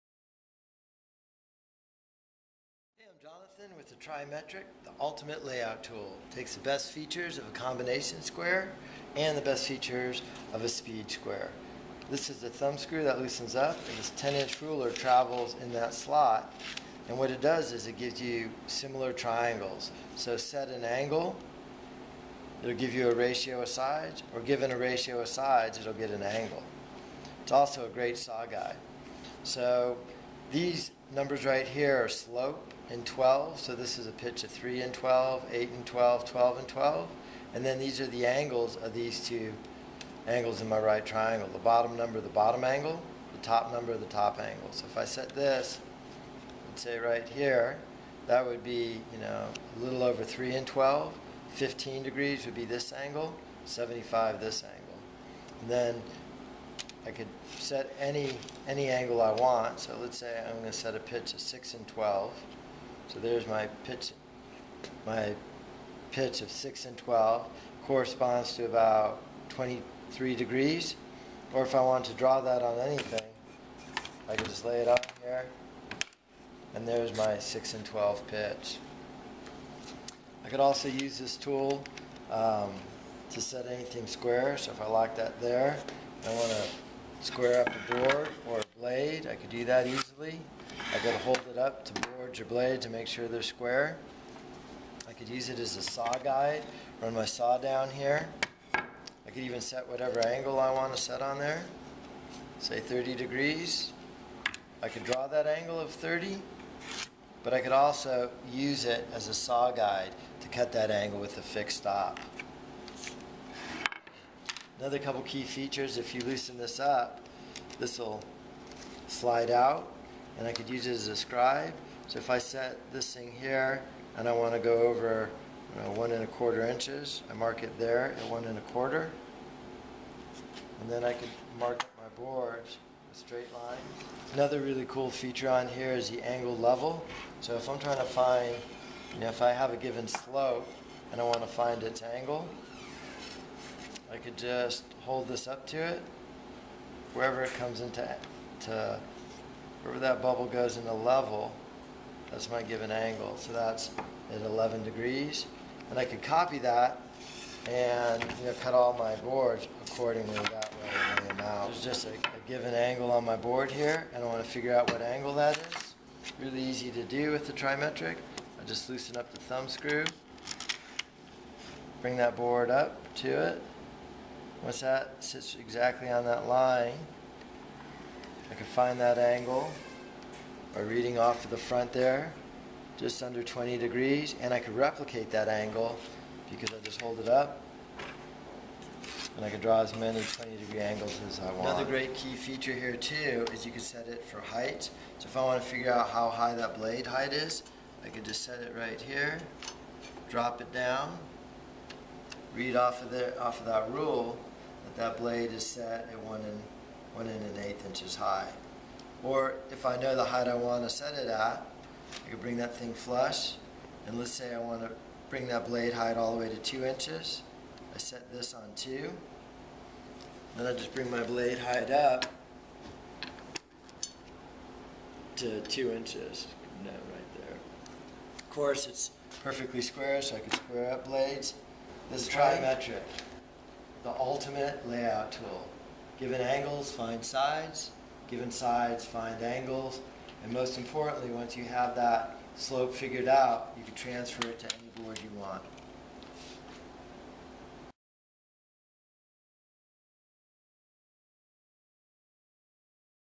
The Tri-Metric
The ultimate lay out tool. Find angles from slopes or given an angle find ratio of sides.  Best of a combination square, best of a speed square.  Saw guide, angled level, bevel all in one.  Watch this video and see the greatest new construction product. Some of my favorite tools